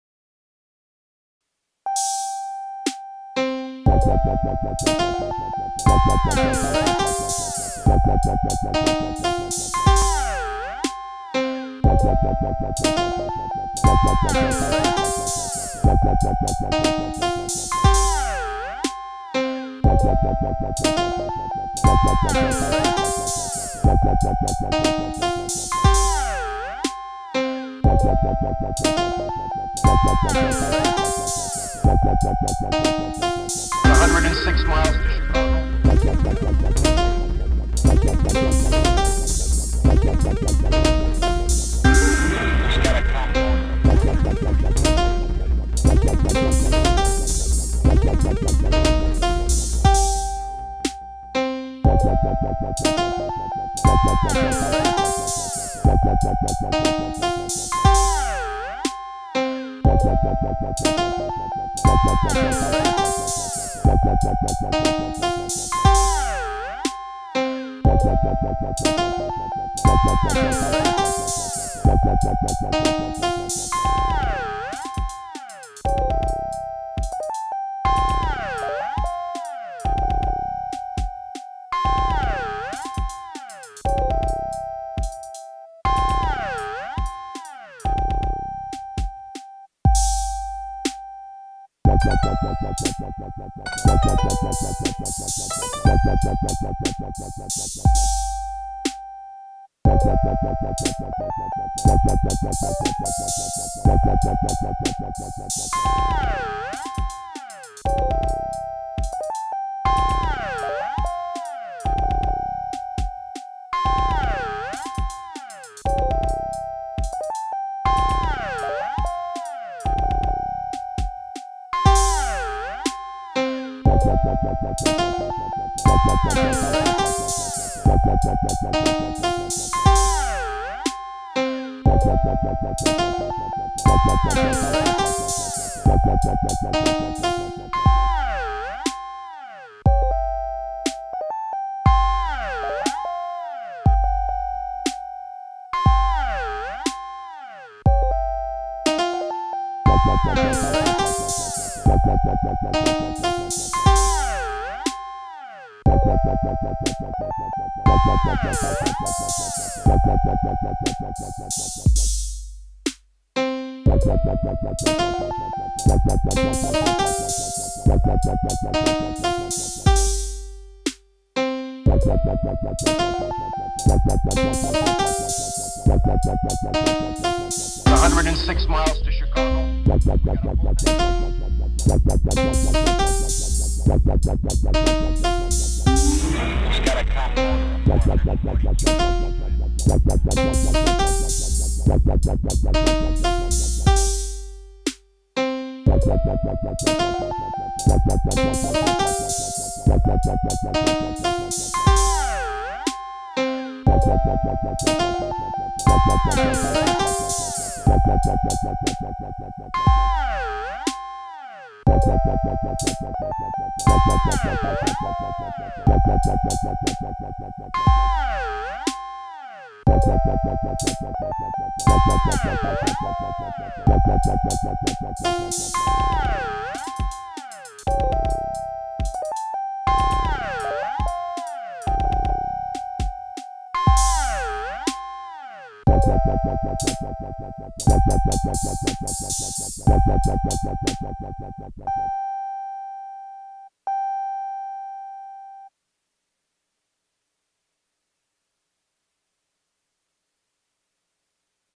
Elektron Model:Samples X2
Midi out of unit left into Kenton Thru 12 then into other unit... Recorded with a Behringer QX2222usb mixer and Audacity...